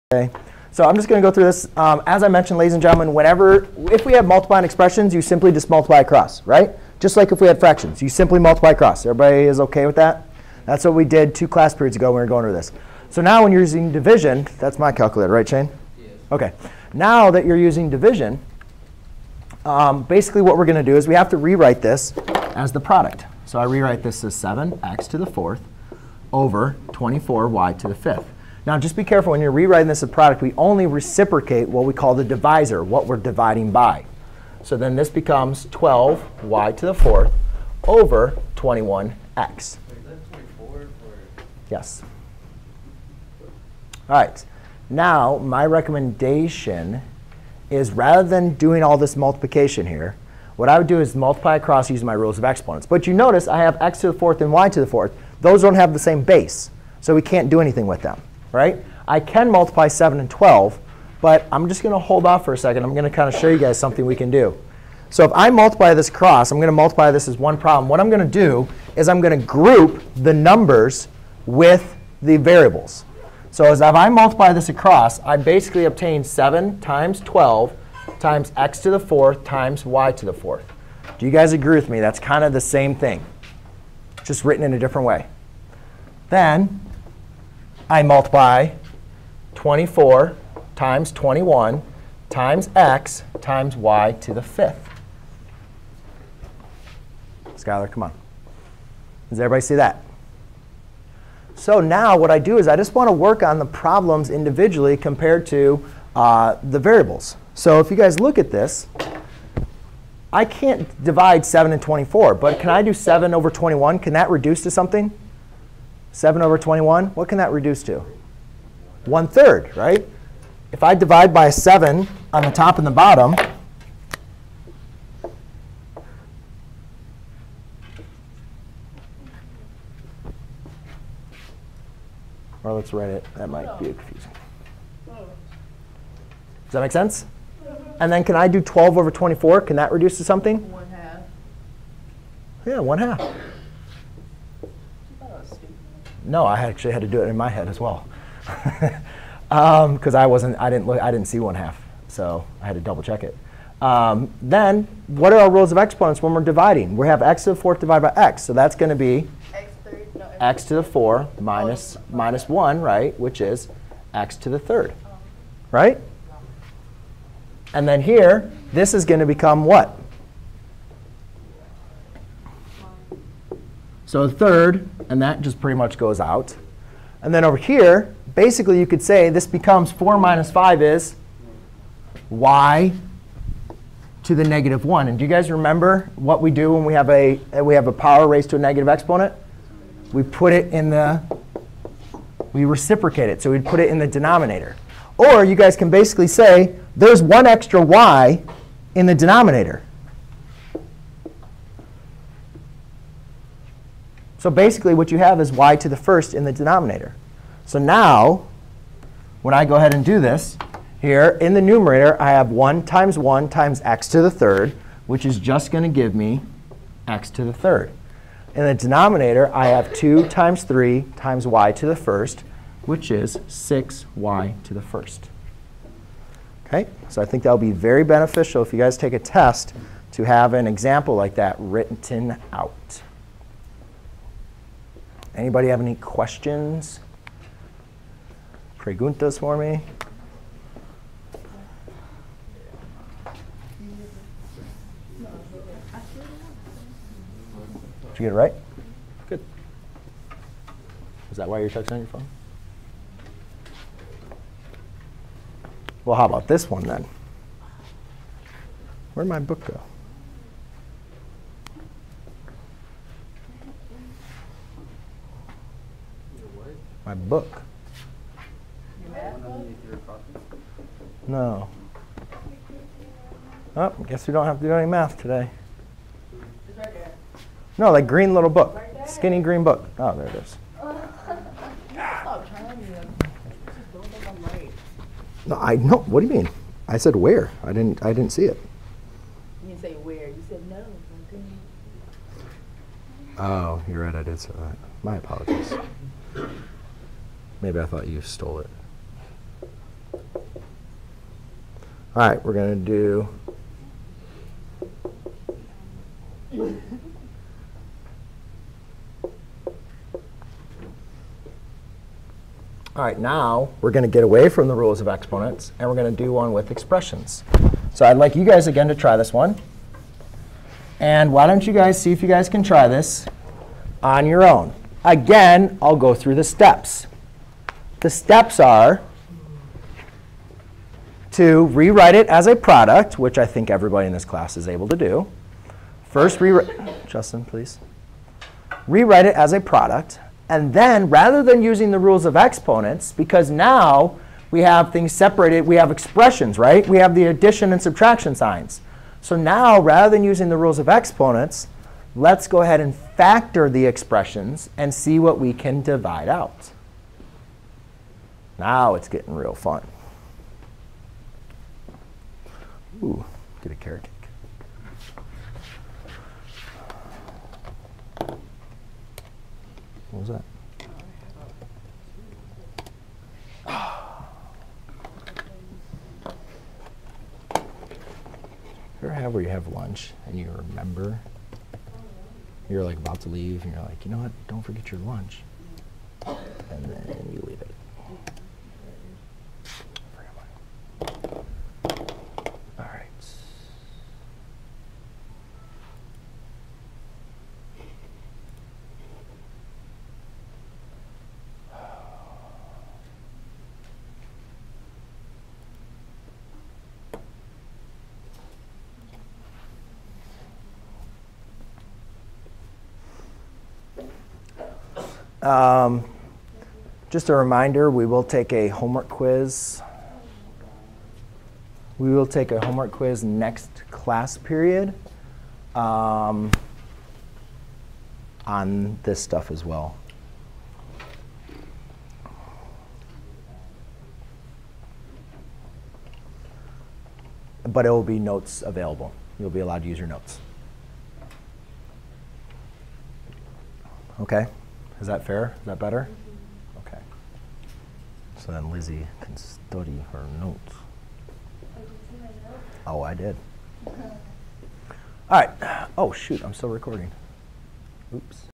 Dividing two rational expressions using the quotient rules of exponents
👉 Learn how to simplify expressions by dividing its terms. Remember that dividing a fraction is the same as multiplying by the reciprocal. When multiplying expressions, each individual term of the expression is multiplied to its like term and the exponents is evaluated using the product rule, the quotient rule or/and the negative exponent rule of exponents.